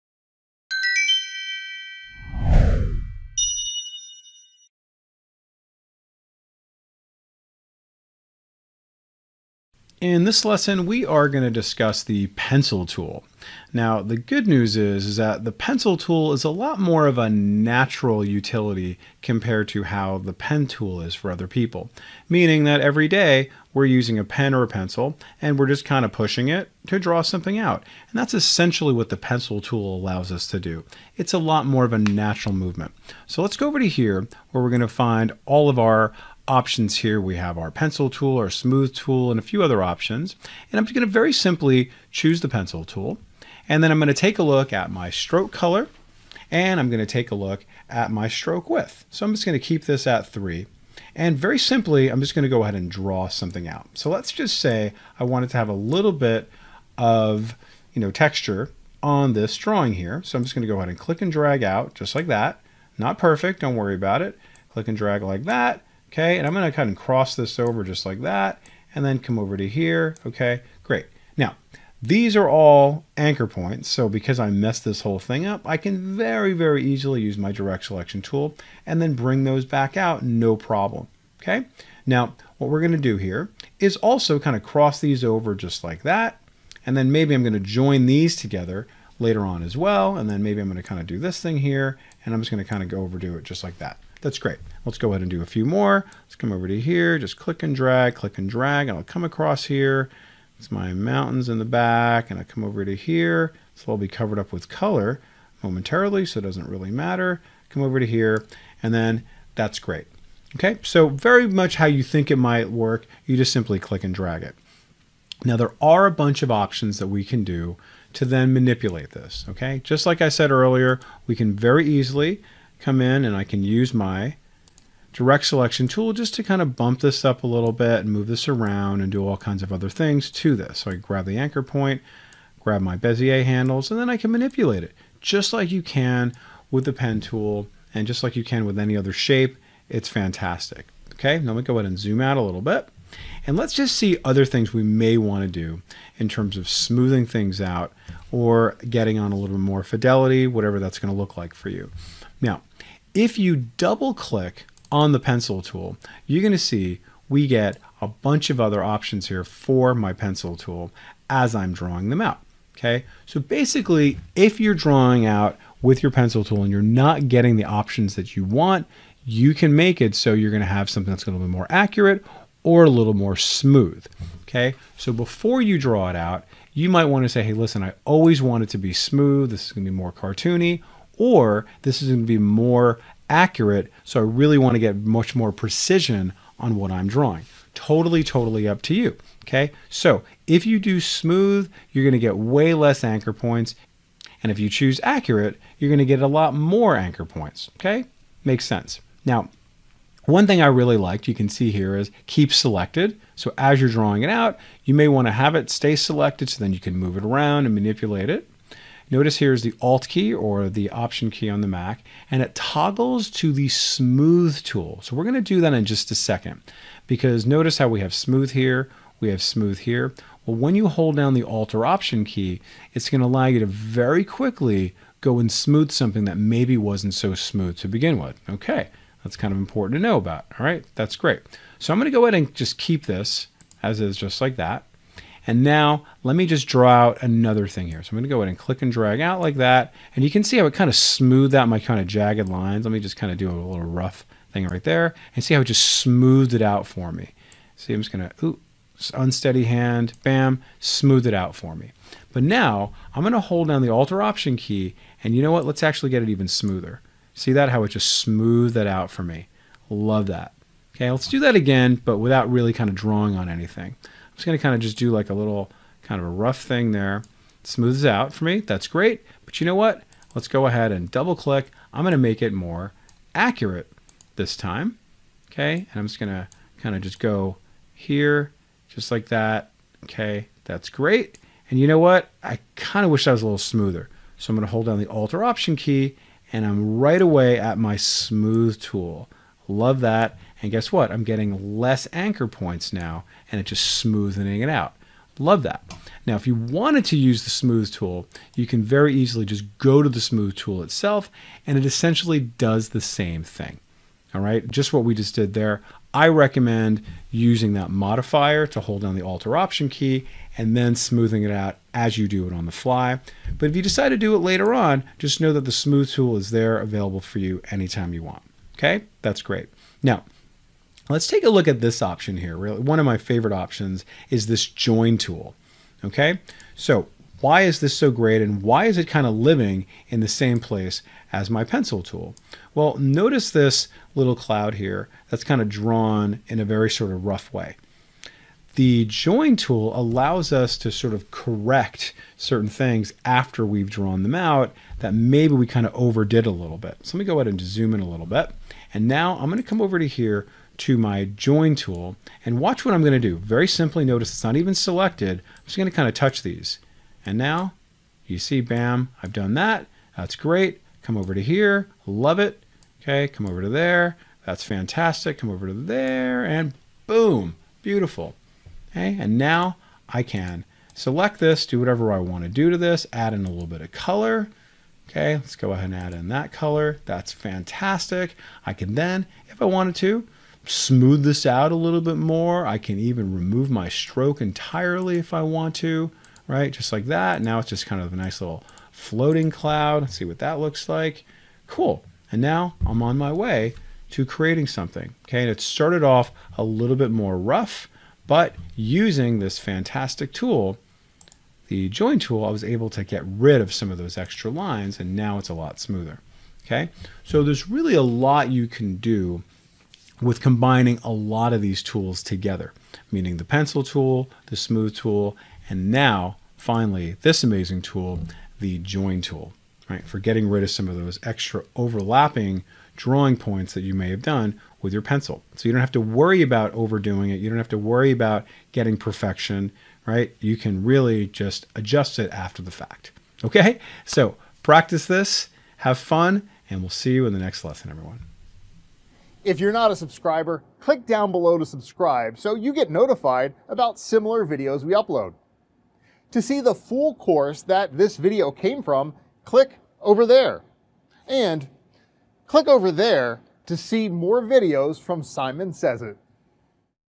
How to Use the Pencil Tool in Adobe Illustrator
Ever felt daunted by the precision demands of the pencil tool? Fear not! In today's lesson, we dive into the versatile Pencil Tool – your gateway to more natural and intuitive drawing experiences. Unlike its meticulous counterpart, the Pencil Tool mimics the familiar feel of sketching with a pencil or pen, making it a preferred choice for many. Follow along as we explore its features, from adjusting stroke width to crafting a textured illustration effortlessly.